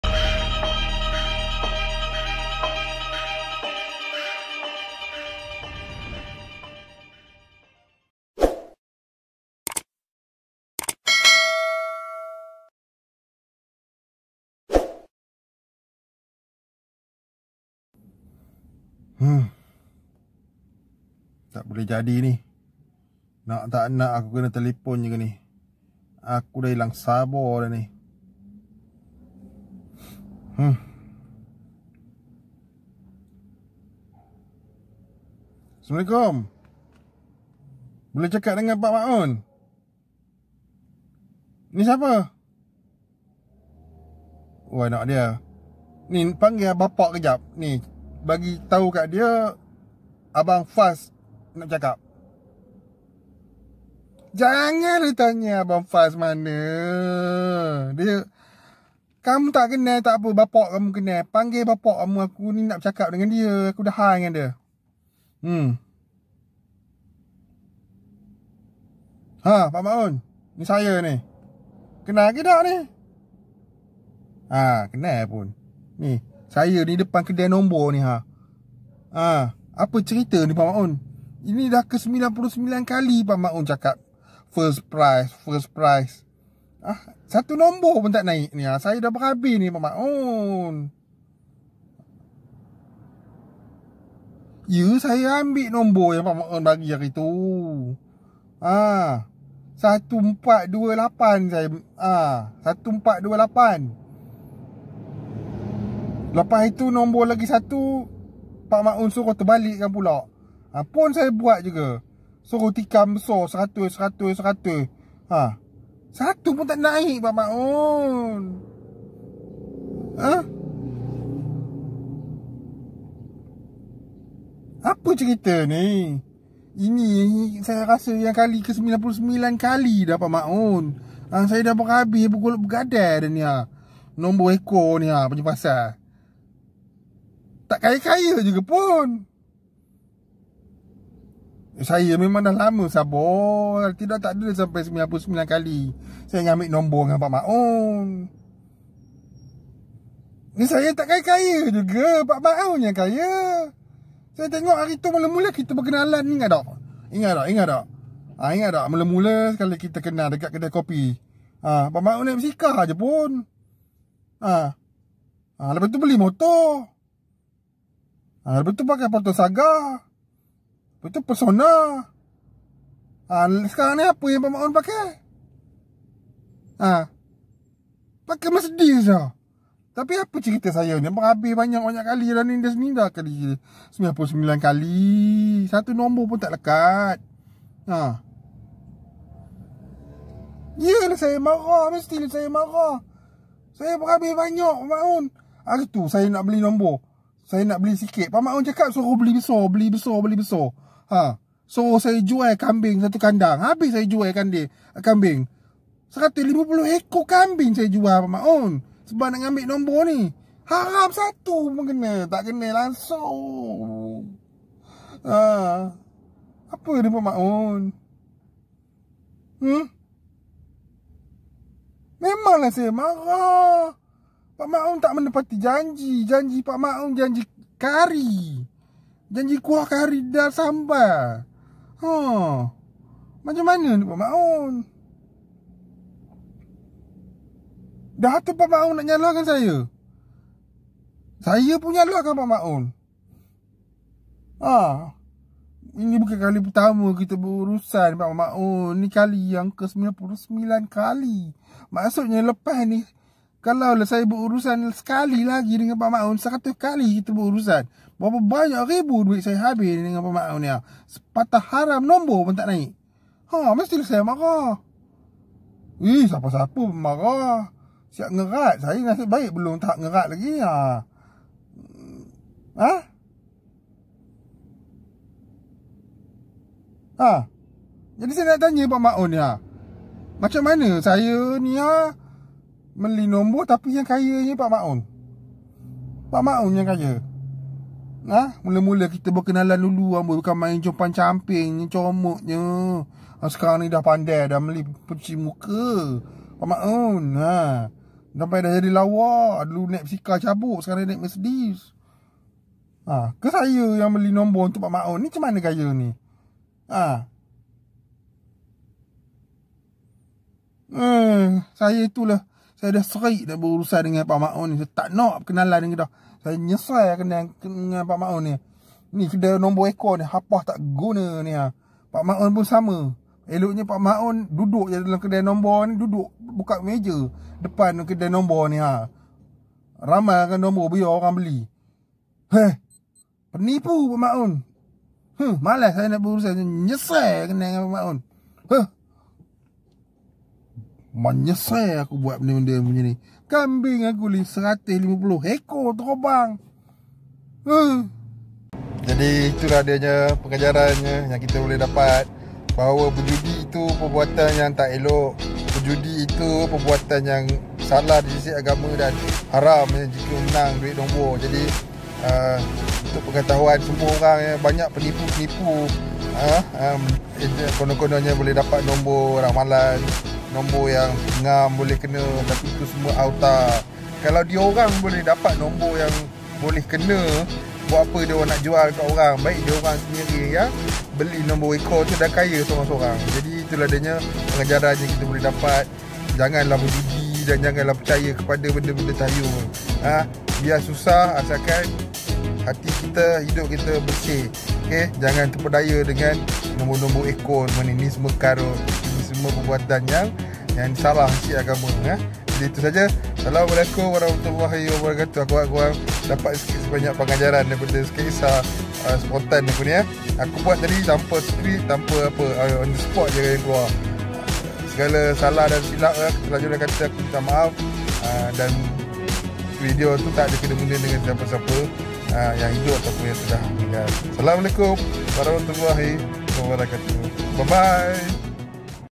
KENA KENCING DENGAN PAK MAUN | BELI NOMBOR EKOR | Sketsa Monolog Spontan. Hadoii..
Pengajaran dan akibat daripada tabiat membeli nombor ekor. Takkan ada manusia yang mampu kekal kaya raya hasil berjudi nombor ekor. Melainkan kena tipu saja. Music